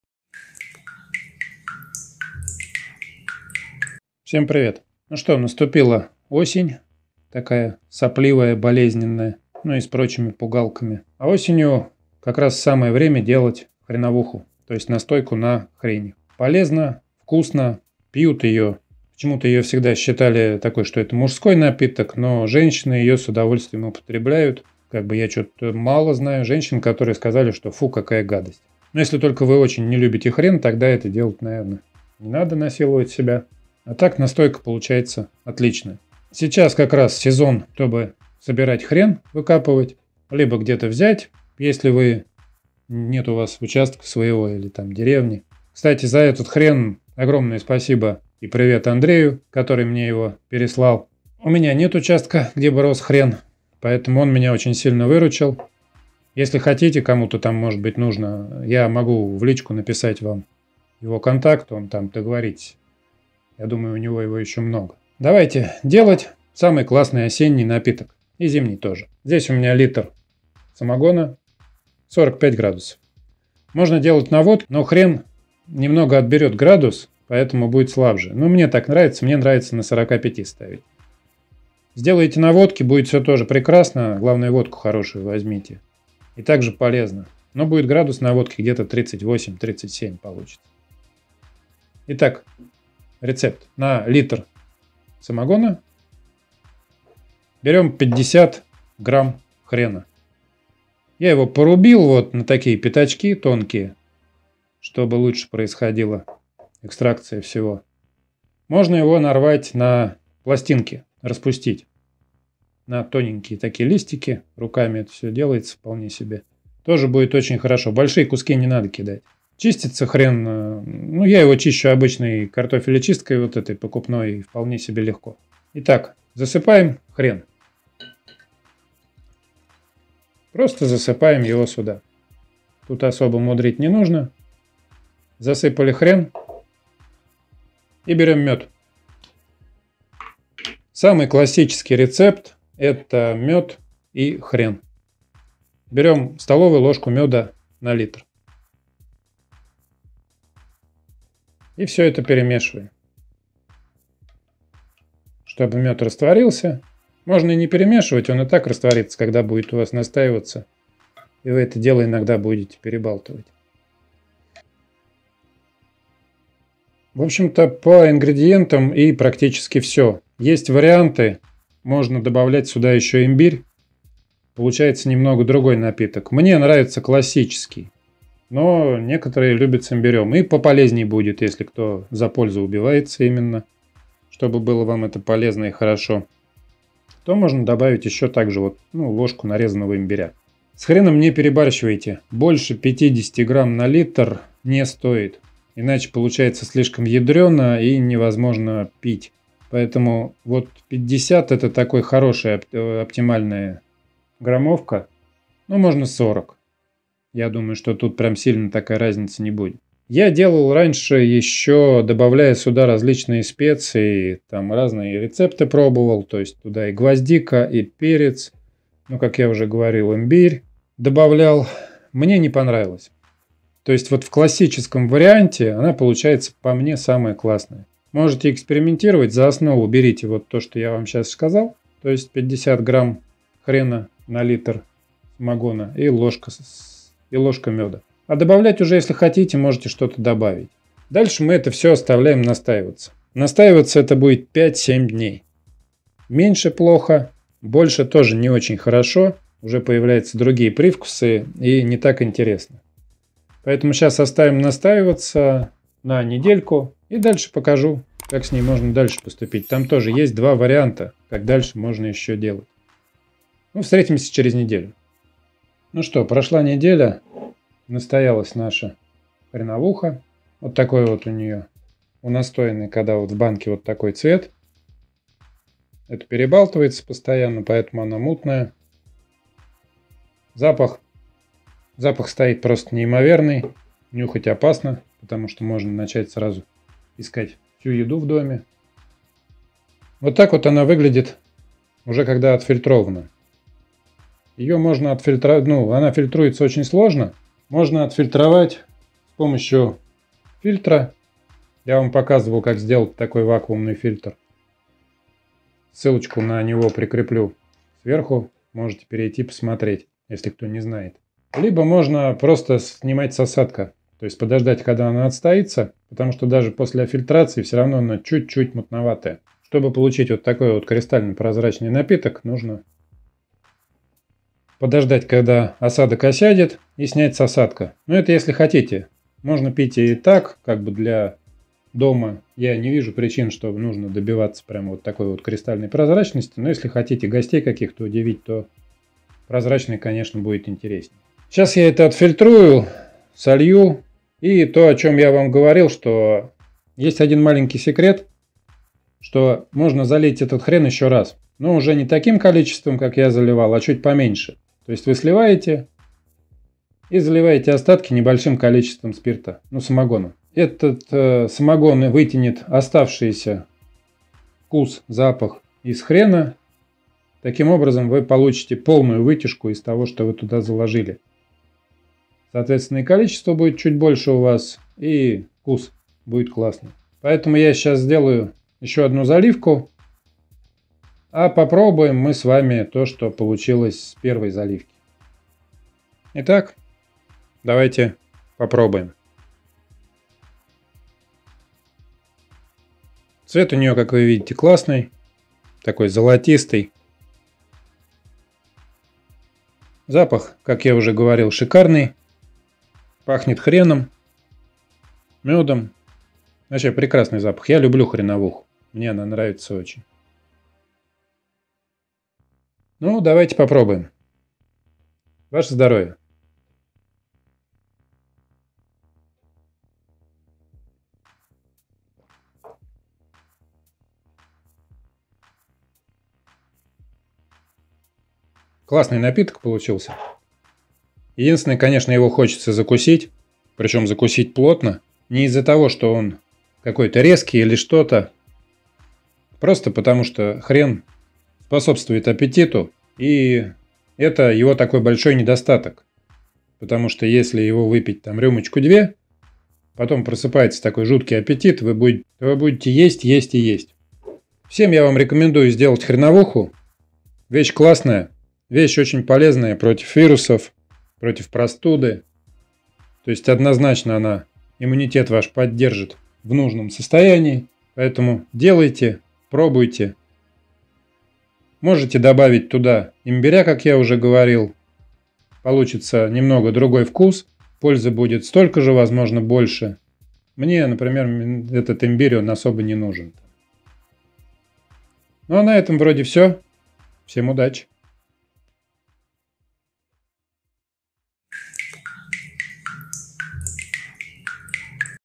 Хреновуха на самогоне. Рецепт домашней хреновухи. Лучшая противовирусная настойка на хрене
Хреновуха на самогоне это самый лучший напиток для осени. Осенью как раз пора выкапывать корень хрена и делать из него настойку на самогоне или водке. Лучшей противовирусной настойки придумать сложно. Рецептов хреновухи очень много, но я вам покажу самый простой рецепт настойки на хрене. Обязательно рекомендую вам сделать хреновуху. Это вкусно, это полезно. Хреновуха повышает иммунитет и настроение

ОГЛАВЛЕНИЕ:
0:00 Ведение. Что такое хреновуха
1:09 Рецепт хреновухи на самогоне
2:50 Варианты рецептов хреновухи
3:43 Время настаивания хреновухи
4:25 Секреты приготовления хорошей хреновухи на самогоне
4:50 Как сделать хреновуху прозрачной и вкусной
6:25 Дегустация настойки на хрене
7:36 Почему нужно пить хреновуху